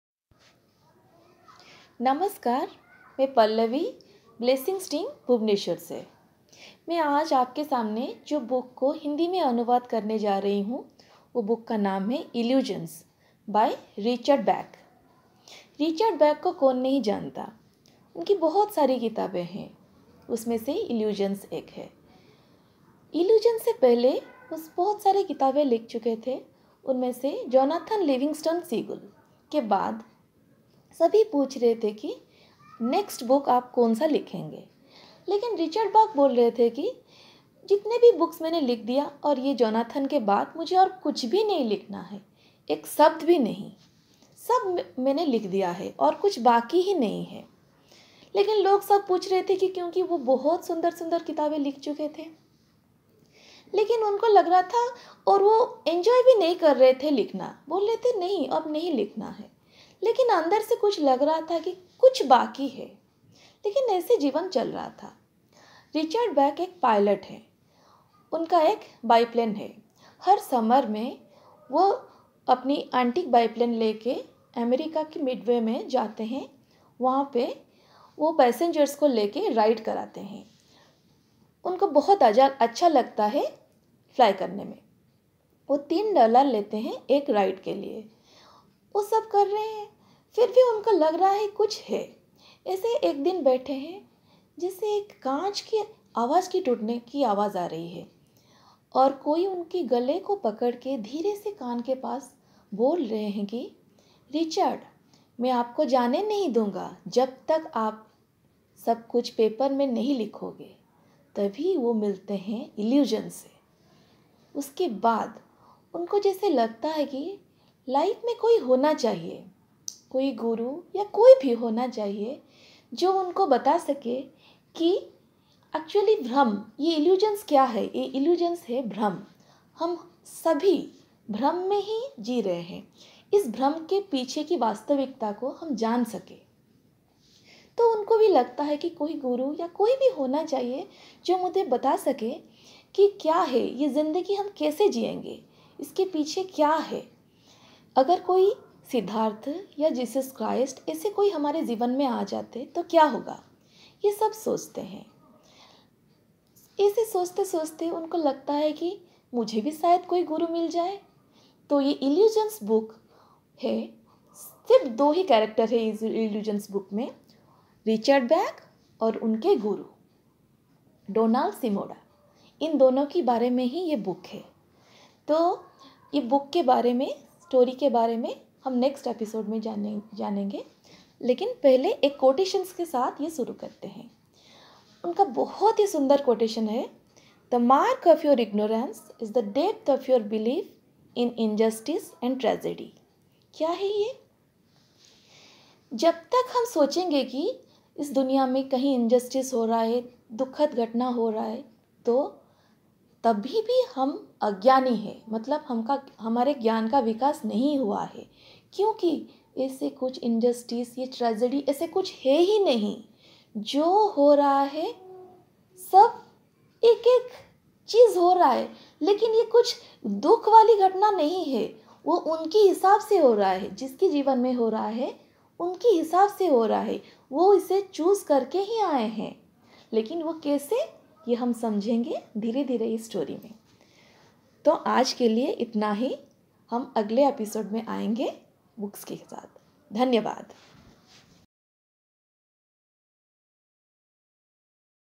Illusions intro.
THIS IS THE HINDI TRANSLATION OF THE BOOK ILLUSIONS WRITTEN BY RICHARD BACH.